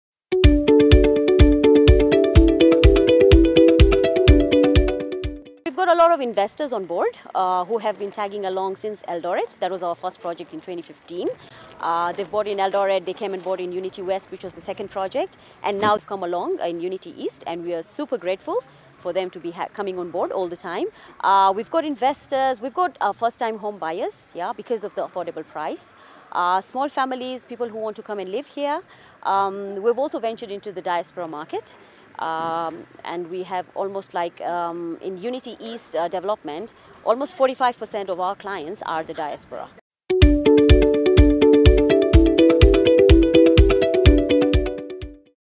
Investing with Unity Homes
Unity Homes Head of sales elaborates growth of the community born out of the families who have invested